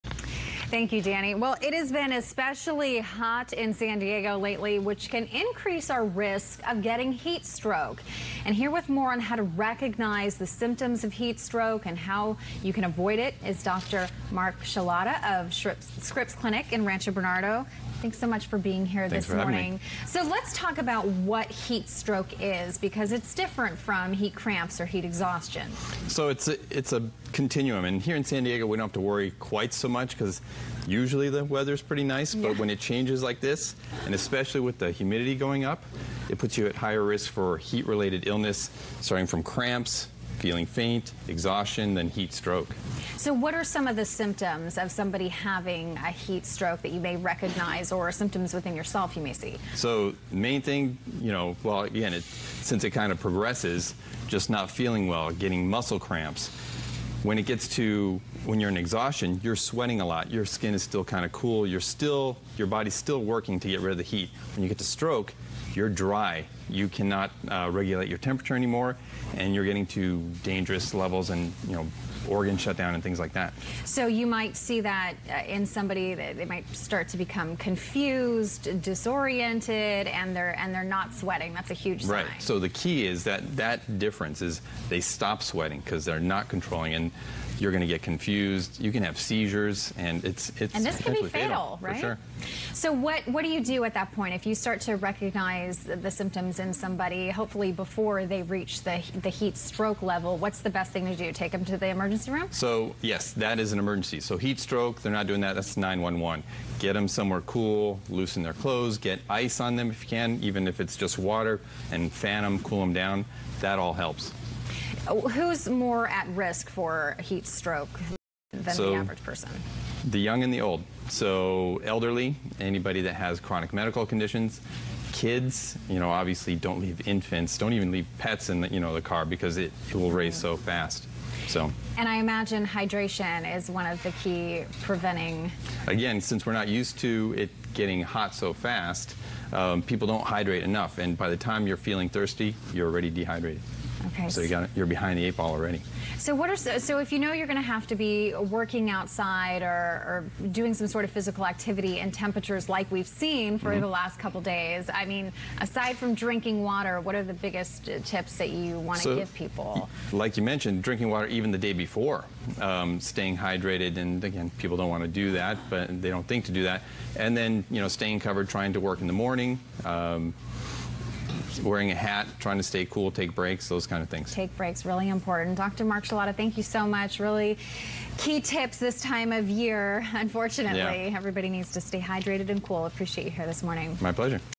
Scripps Health: Understanding Heat Stroke and Prevention with KUSI
Physician Mark Shalauta, MD, explains how to recognize the signs of heat stroke, what you can do to prevent dehydration and how keep your cool with KUSI News.